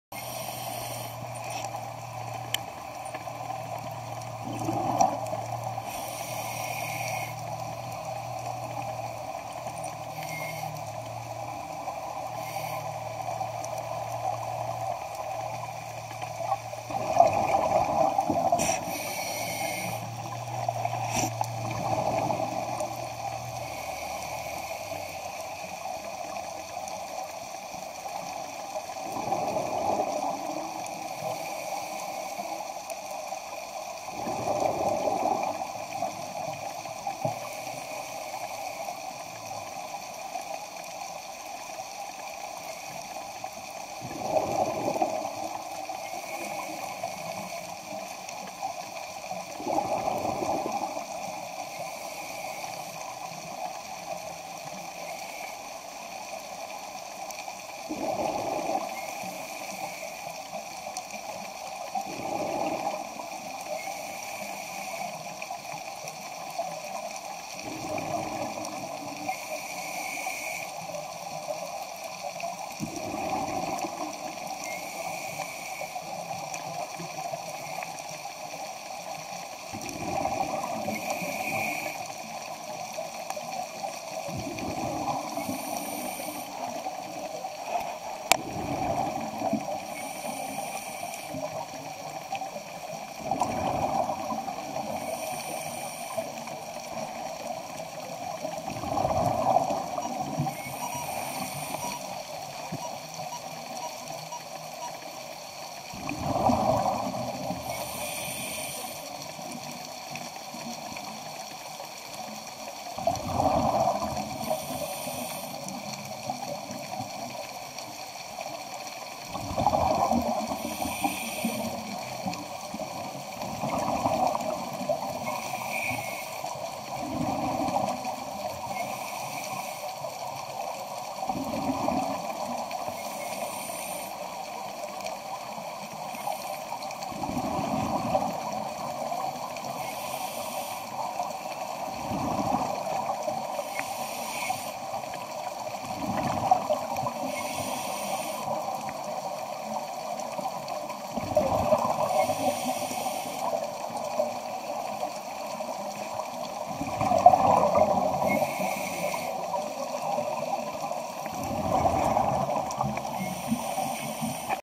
Randy Galapagos Vids 08 (156)WS UTURN.MP4
A curious smaller whale shark that swam past all the divers to me and turned around and swam back past all of us